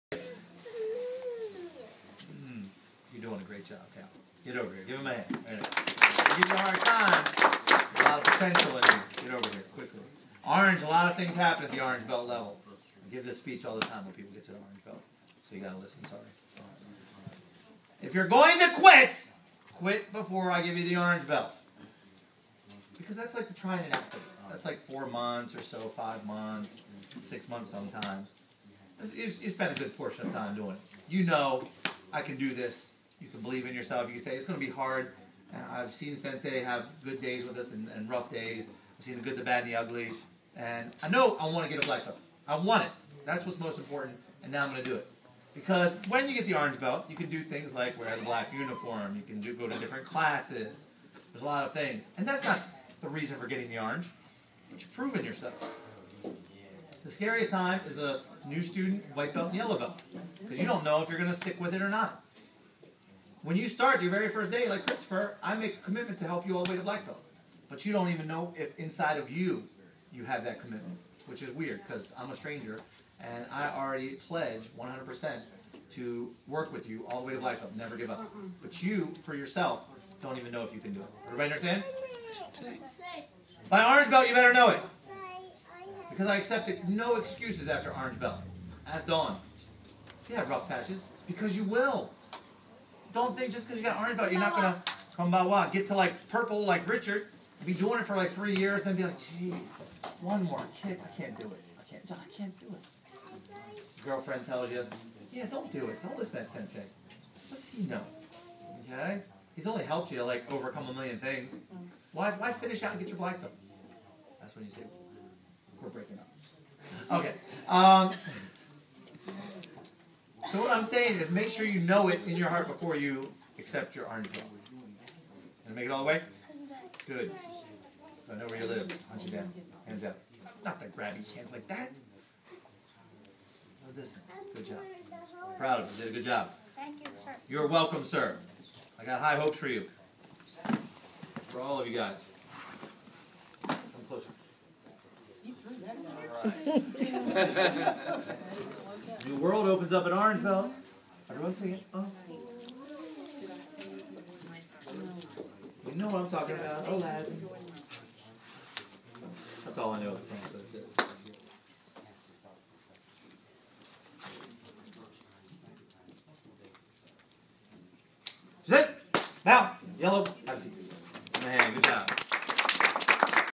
Orange Belt Cal
Cal receives his orange belt from Sensei Dave Street at EMA Family Karate in Essex, MD.